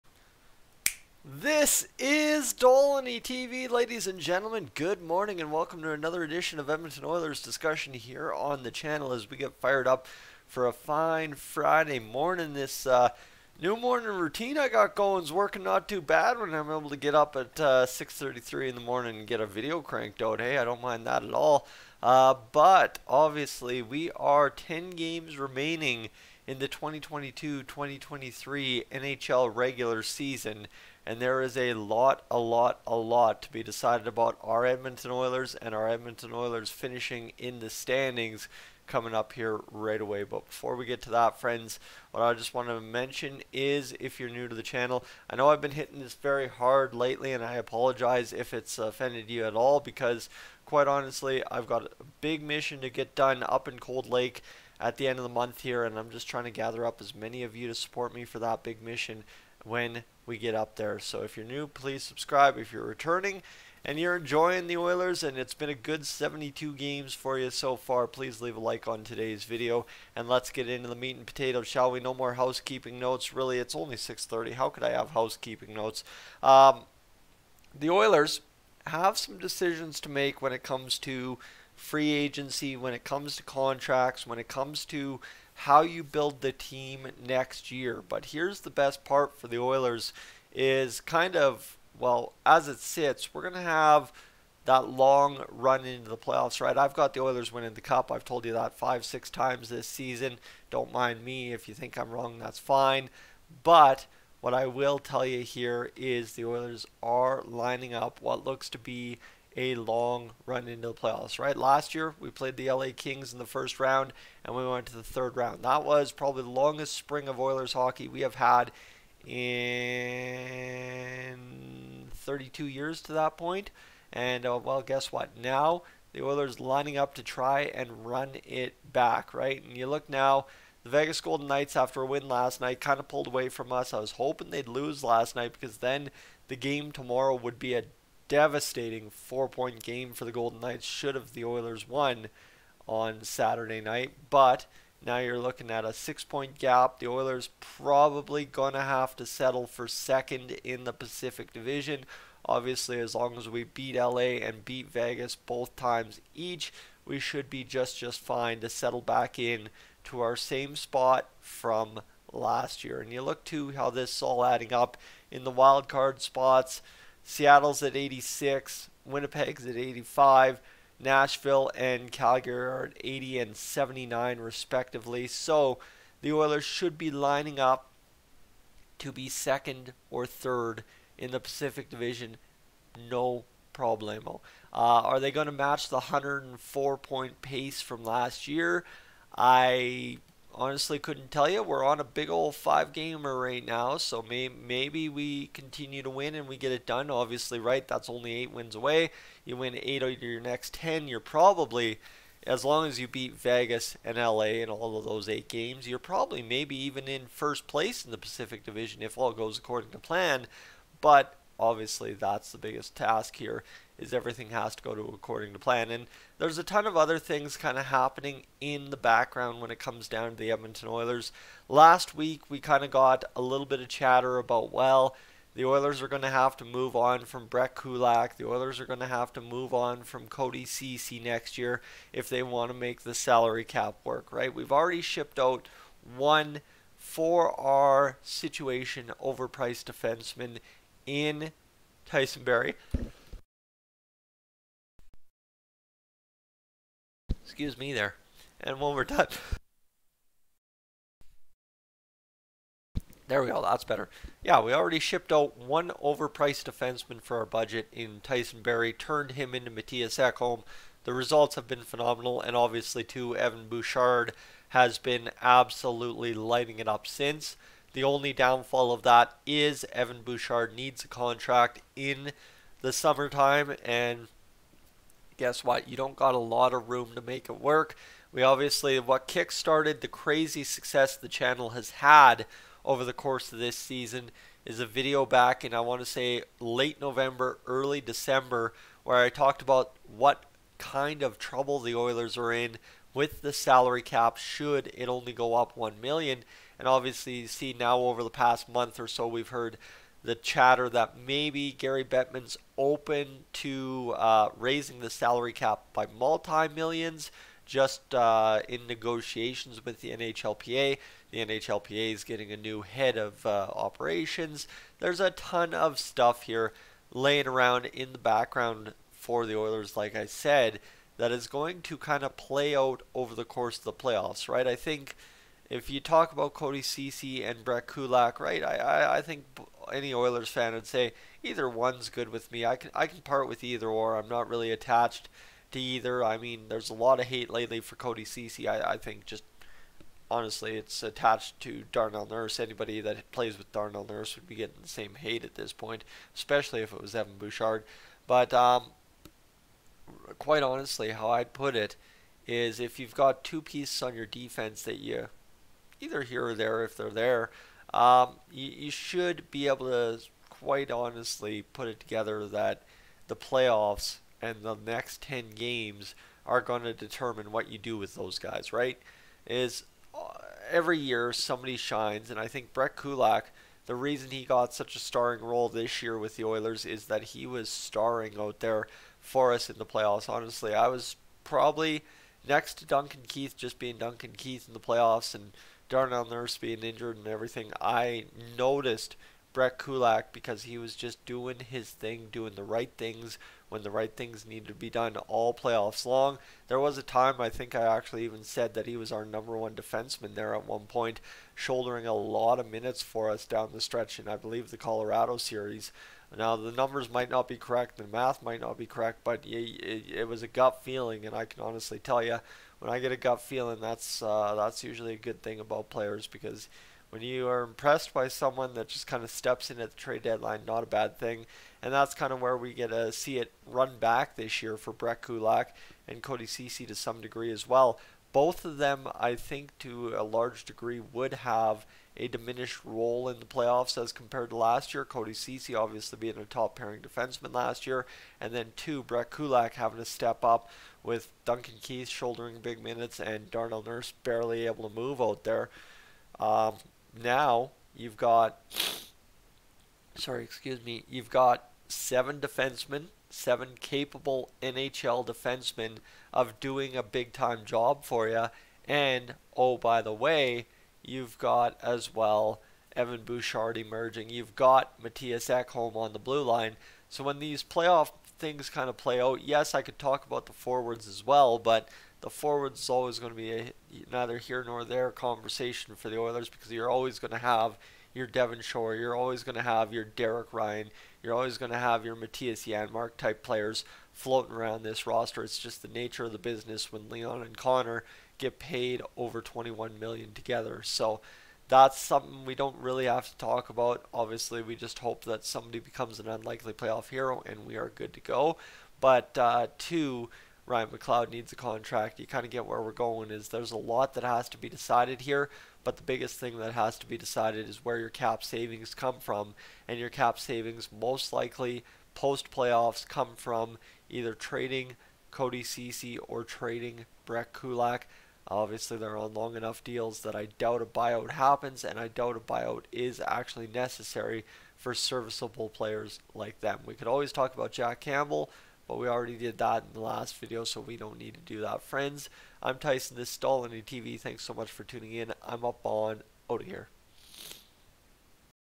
HUGE Stretch Run For The Edmonton Oilers! What Happens Now Determines What Happens Next!
The Edmonton Oilers have a huge stretch run coming up! With some major roster moves needing to be made in the off-season, what happens now will determine what happens next!